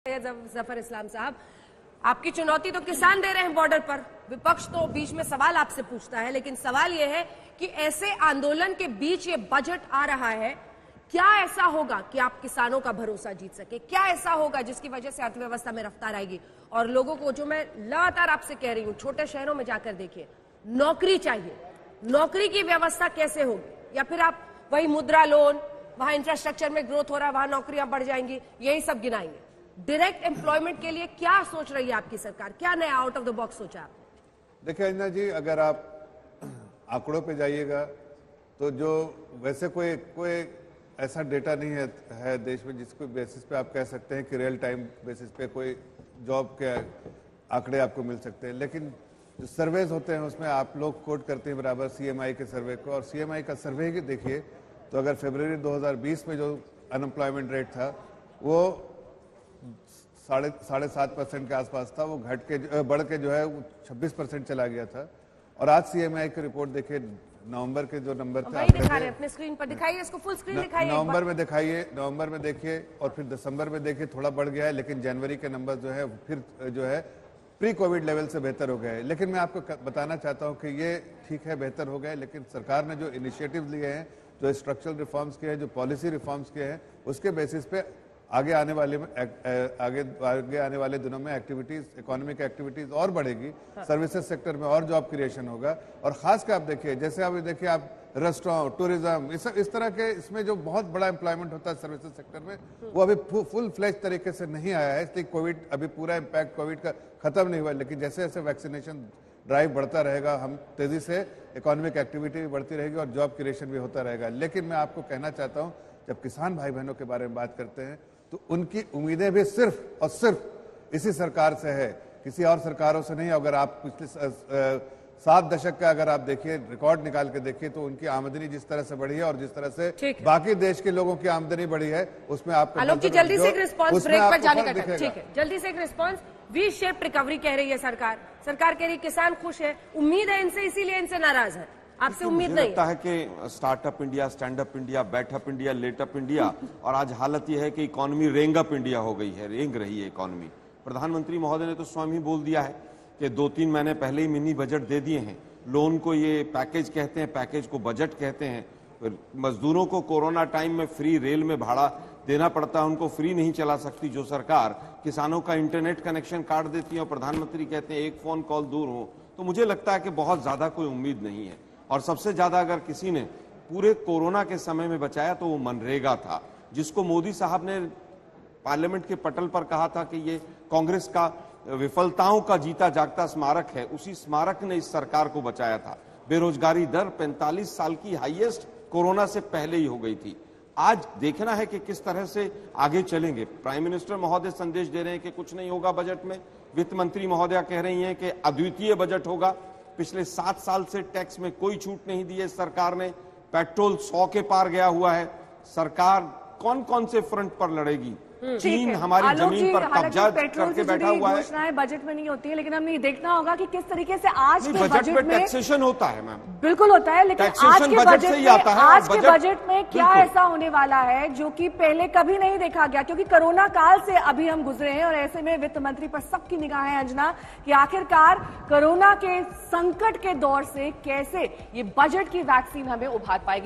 Union Budget 2021-22: Direct Employment के लिए क्या कर रही है सरकार, सुनिए BJP प्रवक्ता का जवाब
Union Budget 2021-22: आज देश का बजट सुबह 11 बजे वित्त मंंत्री Nirmala Sitharaman पेश करेंगे. बजट में ऐसा क्या होगा जो किसानों को भरोसा जीत लेगा.साथ ही Direct Employment को लेकर सरकार का क्या प्लान है. इस पर सुनिए BJP प्रवक्ता का जवाब.